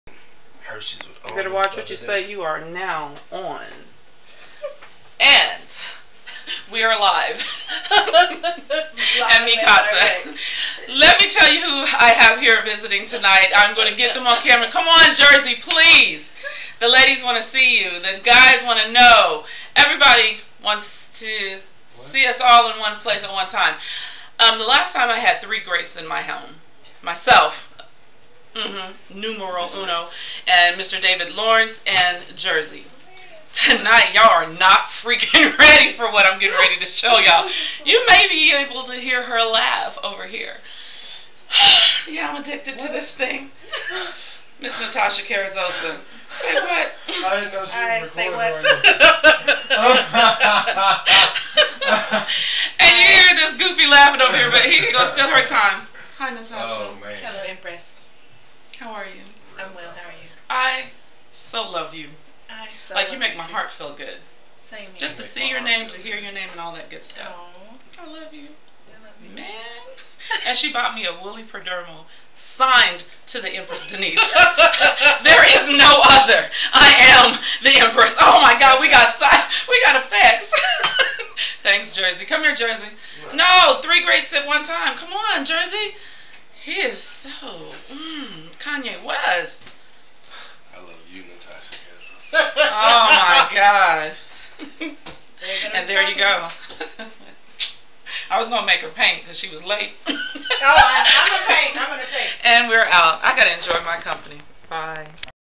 A visit with The Poets Natasha Carrizosa
The One and Only Natasha Carrizosa, William Jersey Daughtry and me Denise D. Cooper (Whitmore Art)
I called us the greats because we are all artist in our own right. Natasha however, is a well known amazing Poet, writer, teacher, mother, host of Natty Roots and so much more. We love her. I miss our talks. Excuse my enthusiasm in this video. I adore her.
Poets and Painters